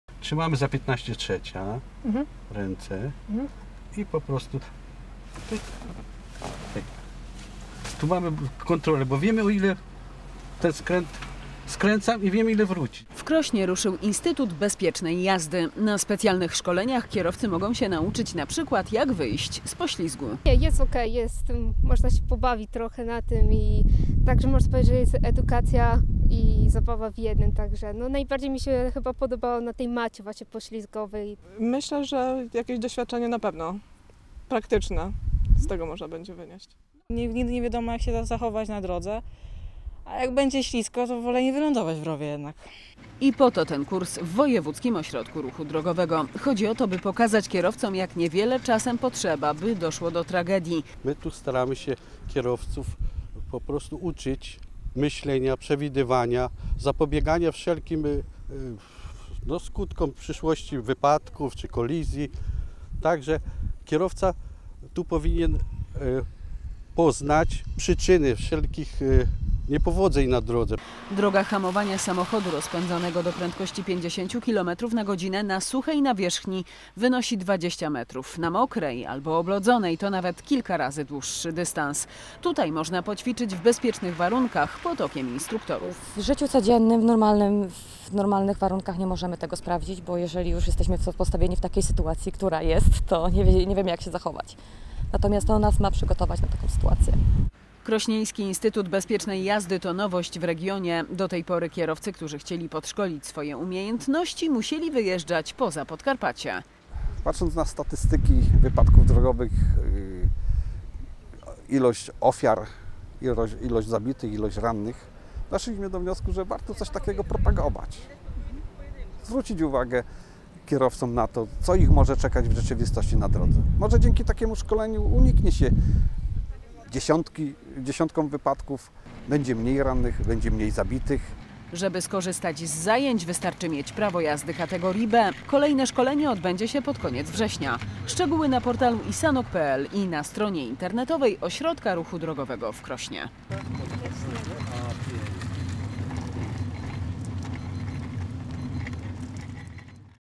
W Krośnie działa Instytut Bezpiecznej Jazdy. Sprawdź się w ekstremalnych warunkach
KROSNO. Są jeszcze wolne miejsca na wrześniowe szkolenie w Instytucie Bezpiecznej Jazdy w Krośnie. Tutaj nauczysz się wychodzić z poślizgu, dodatkowo udoskonalisz technikę kierowania. Jeśli chcesz sprawdzić swoje umiejętności w ekstremalnych warunkach, zapisz się!
Szkolenie trwa około trzech godzin i obejmuje naukę teorii oraz zajęcia praktyczne, m.in. hamowanie awaryjne na śliskiej nawierzchni i symulator zderzeń. - Mata poślizgowa to u nas nowość. Dzięki niej kierowcy mogą sprawdzić swoje umiejętności w bezpiecznych warunkach. Kiedy na drodze wpadamy w poślizg, na błędy nie ma miejsca, bo możemy przypłacić je życiem. A trzeba pamiętać, że często na decyzję i reakcję mamy jedynie ułamki sekund -- tłumaczy Ewelina Nechrebecka z WORD Krosno. Więcej na